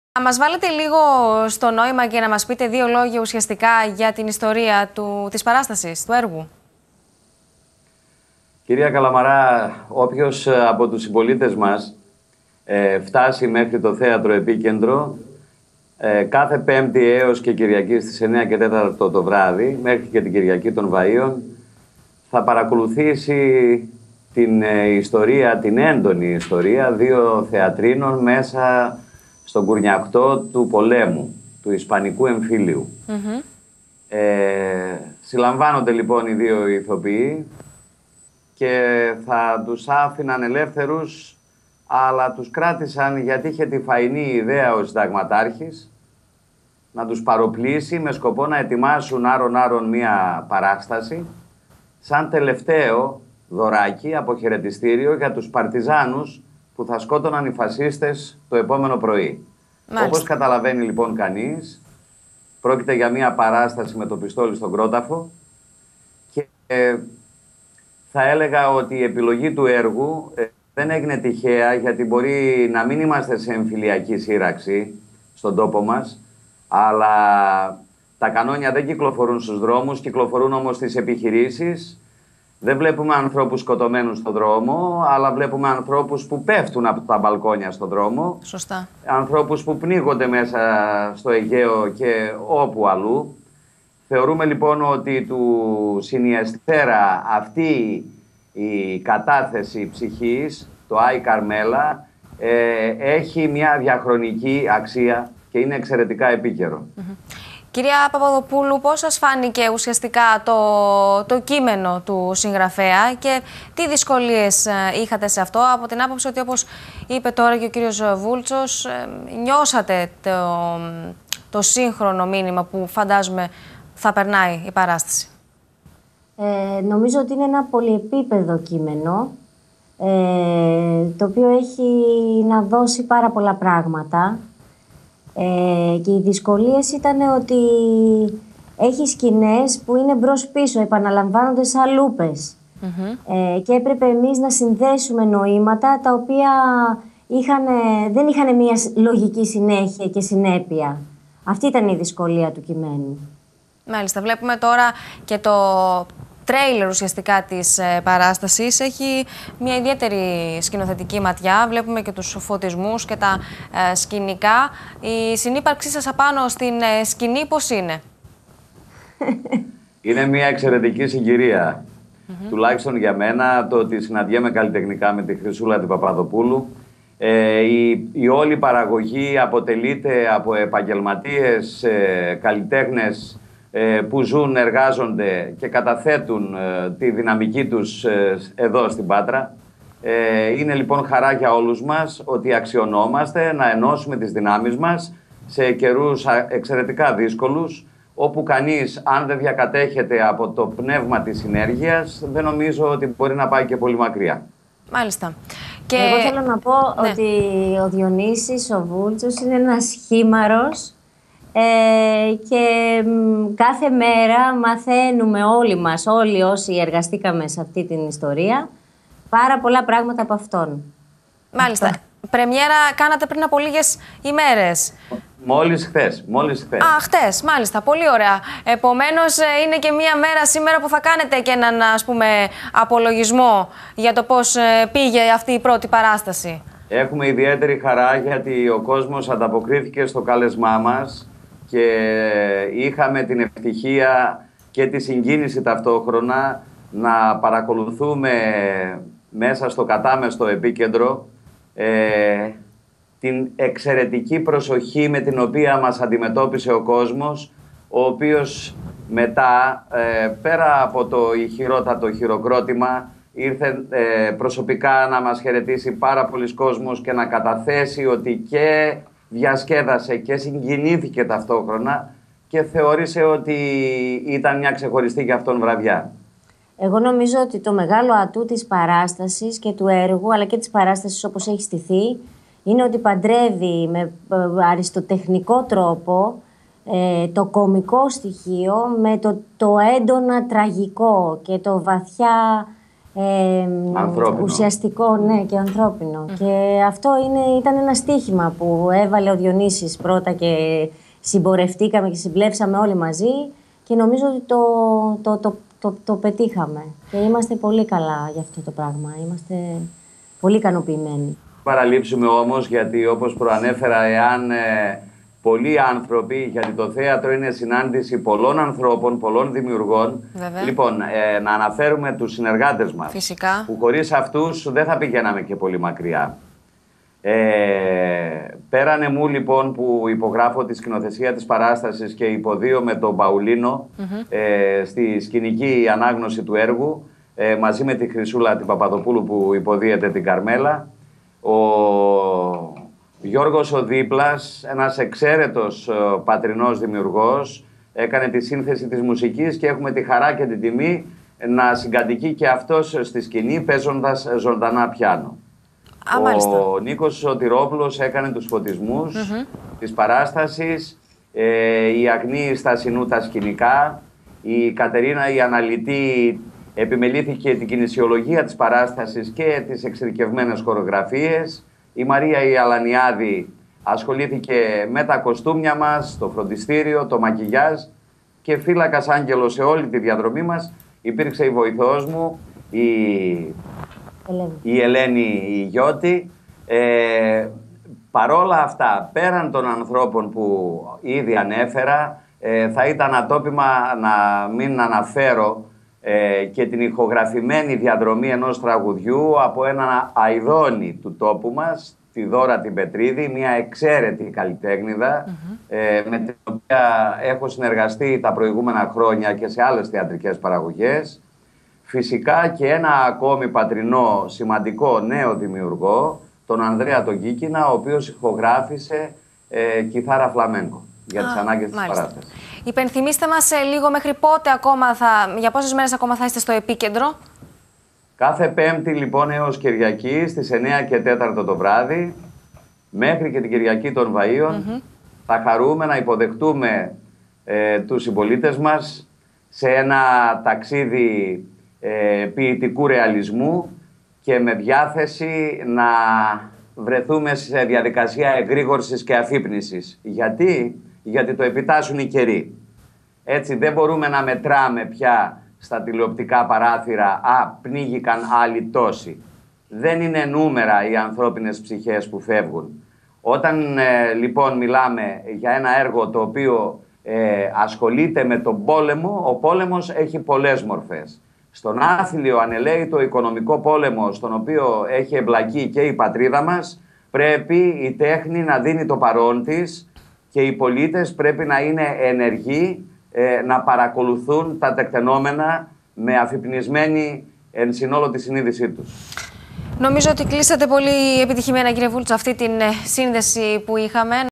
Πάτρα: Νέα θεατρική παραγωγή του Επίκεντρο+ «Ay, Carmela!»
«Ay, Carmela!» του ισπανού José Sanchís Sinisterra στο Επίκεντρο+
Χρυσούλα Παπαδοπούλου – Ηθοποιός
Διονύσης Βούλτσος – Ηθοποιός